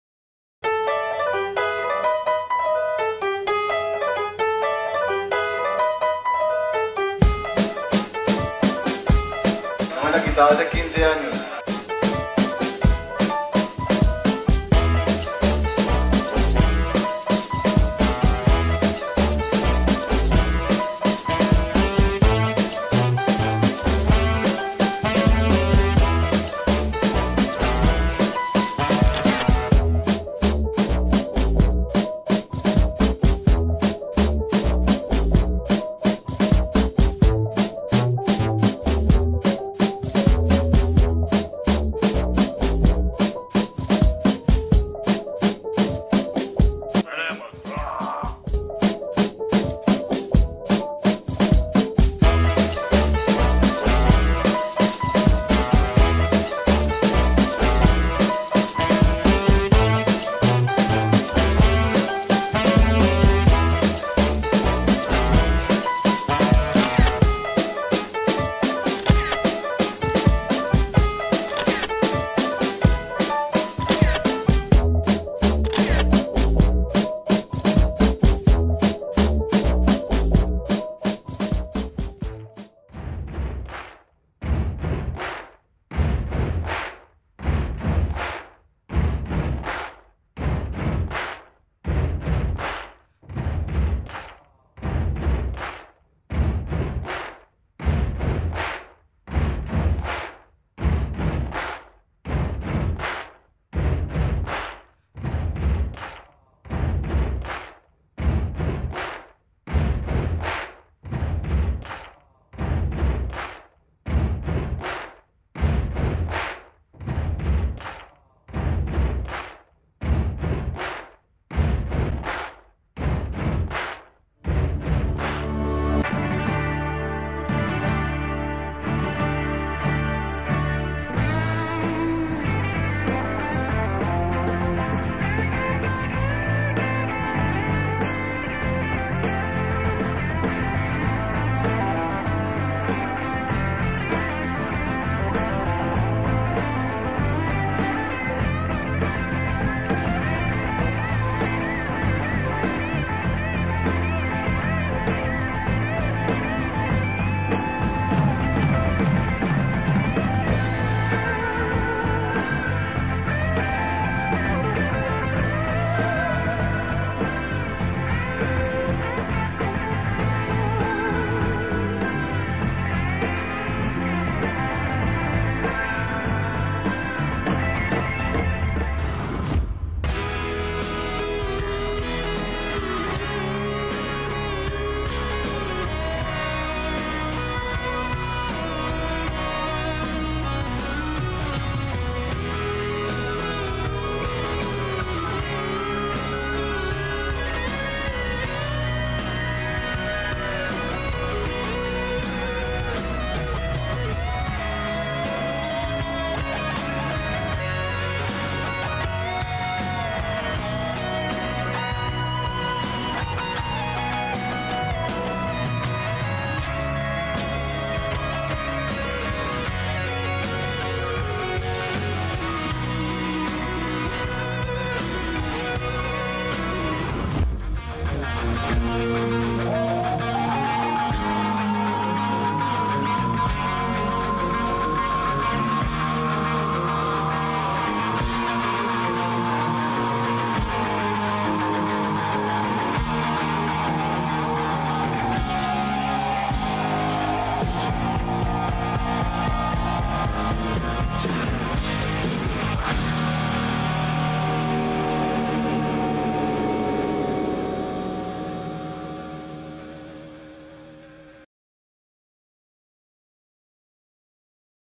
Disfrázate con Vibra - Mercadeo y Tecnica - Ochentas
Las áreas Mercadeo y Tecnica de Radiopolis se disfraza de Ochentas, disfrázate con vibra.